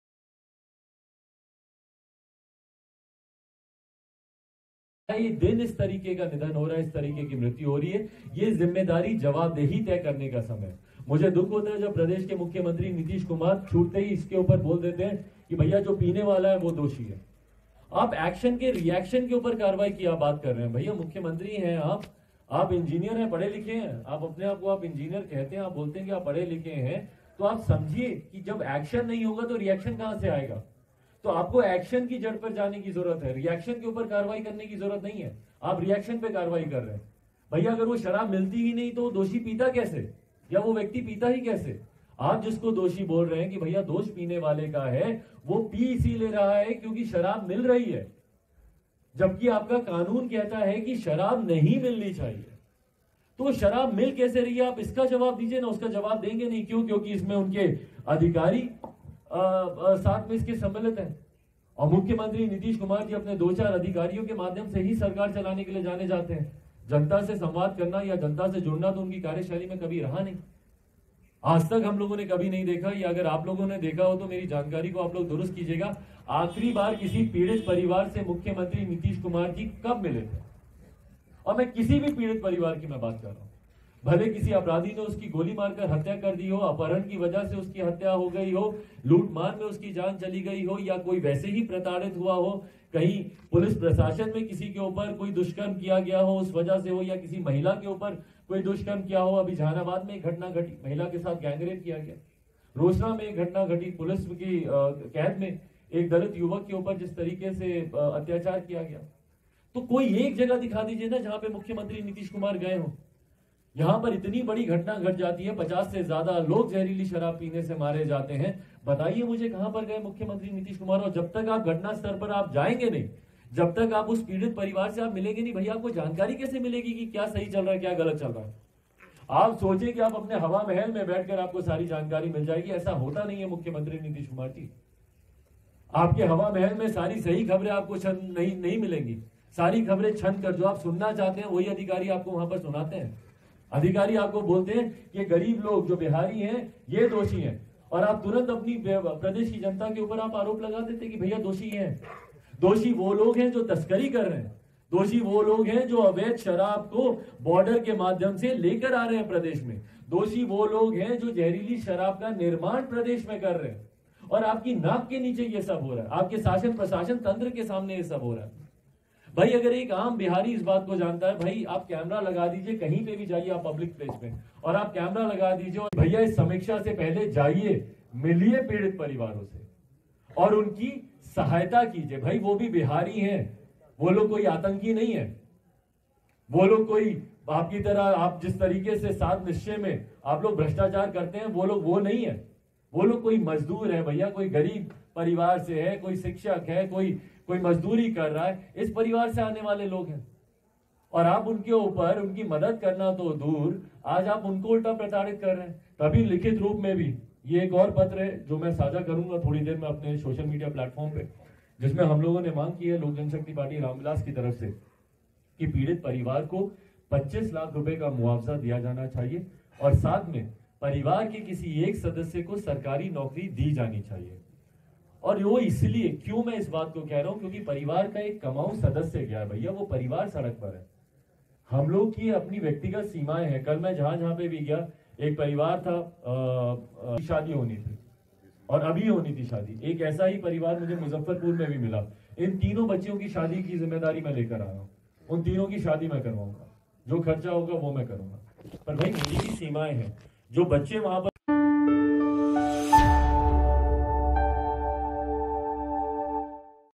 'Bihar में कैसे मिल रही शराब, जवाब दें CM', शराबकांड को लेकर Chirag Paswan का Nitish Kumar पर निशाना
पटना। बिहार में जारी शराबबंदी और जहरीली शराब से हो रही लगातार मौतों पर लोजपा (रामविलास) के अध्यक्ष चिराग पासवान ने निशाना साधा है। चिराग ने  कहा कि 2016 में मुख्यमंत्री नीतीश कुमार शराबबंदी कानून को लेकर आये थे, हमलोग विपक्ष में थे और हमलोग ने इस कानून का समर्थन किया था। लेकिन शराबबंदी कानून पूरी तरह से घरातल पर नहीं दिख रहा है। चिराग ने कहा कि ज़हरीली शराब से कई मौत हुई हैं, मुख्यमंत्री नीतीश कुमार को एक्शन लेने की जरूरत है। सीएम नीतीश कुमार इस बात का जवाब दें कि आखिर बिहार में शराब कैसे मिल रही है। उन्होंने कहा कि नीतीश कुमार कभी किसी पीड़ित से मिलने नहीं गए हैं। शराबकांड के लिए जनता दोषी नहीं, दोषी वो लोग हैं, जो शराब लाते हैं।